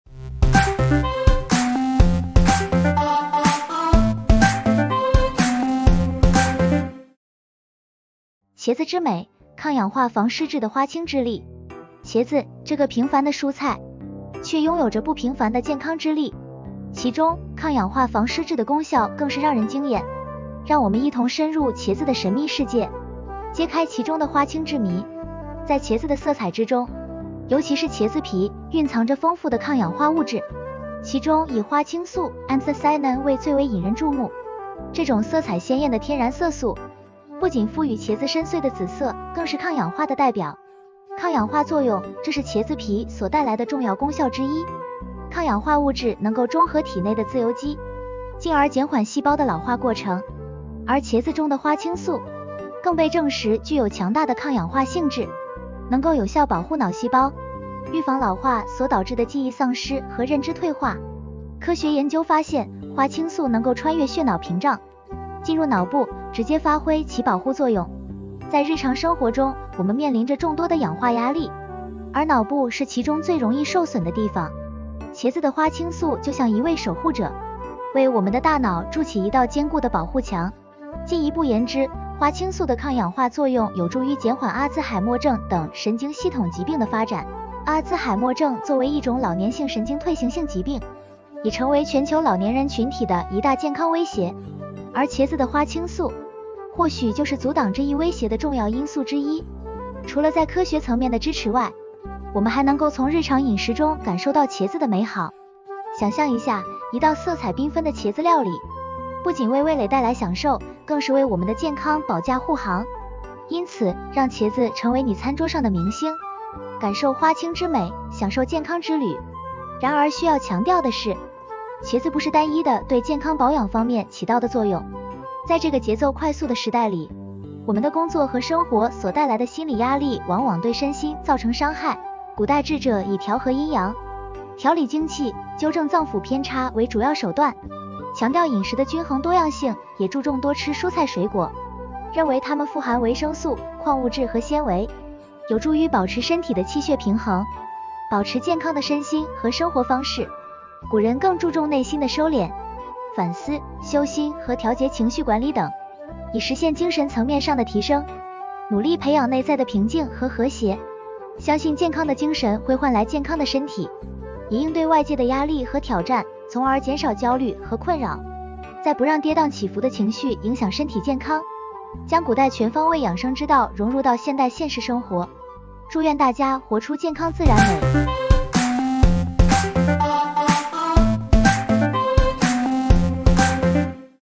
【大腦 保護】補腦 超級食物：茄子- 抗氧化 防失智的花青素 ，大腦 保護的秘密武器 ！
【大腦 保護】補腦 超級食物：茄子- 抗氧化 防失智的花青素 ，大腦 保護的秘密武器 ！|補腦 吃什麽？
- 抗氧化防失智的花青之力- 茄子，這個平凡的蔬菜，卻擁有著不平凡大腦 保護的健康之力。其中，抗氧化防失智的功效更是讓人驚艷。讓我們一同深入茄子的神秘世界，揭開其中的花青之謎。
茄子含有豐富的花青素，這是一種強效的抗氧化物質，具有抗氧化和抗發炎的功效。花青素是茄子皮顏色的主要成分，其顏色範圍從深紫到鮮艷的紅色，正是這些色素賦予了茄子多變的外觀，同時也是其大腦 保護的秘密武器。

抗氧化作用是花青素最引人矚目的特性之一。在人體內，自由基的過量生成可能導致氧化損傷，進而引發多種疾病，包括老年癡呆症。而花青素能夠穩定自由基，減少對神經細胞的損害，進而保護大腦健康，延緩認知功能衰退的發展。除了抗氧化作用，花青素還具有抗發炎的作用。炎症反應在某種程度上是身體對抗外來侵害的自然反應，但長期的慢性炎症可能對大腦功能造成損害，甚至加速認知功能的衰退。花青素通過抑制炎症反應中的一些關鍵分子，如腫瘤壞死因子α（TNF-α）和白介素-1β（IL-1β），有助於減輕炎症對大腦 保護的影響。補腦 超級食物：茄子- 抗氧化 防失智的花青素 ，大腦 保護的秘密武器 ！

然而需要強調的是，茄子不是單一的保護 大腦對健康保養方面起到的作用。在這個節奏快速的時代裡，我們的工作和生活所帶來的心理壓力往往對身心造成傷害。古代智者以調和陰陽、調理精氣、糾正臟腑偏差為主要手段，強調飲食的均衡多樣性，也注重多吃蔬菜水果，認為它們富含維生素、礦物質和纖維，有助於保持身體的氣血平衡，保持健康的身心和生活方式。 古人更注重内心的收斂，反思，修心和調節情緒管理等，以實現精神層面上的提升，努力培養內在的平靜和和諧，相信健康的精神會換來健康的身體，以應對外界的壓力和挑戰，從而減少焦慮和困擾，再不讓跌蕩起伏的情緒影響身體健康，將古代全方位養生之道融入到現代現實生活，祝願大家活出健康自然美！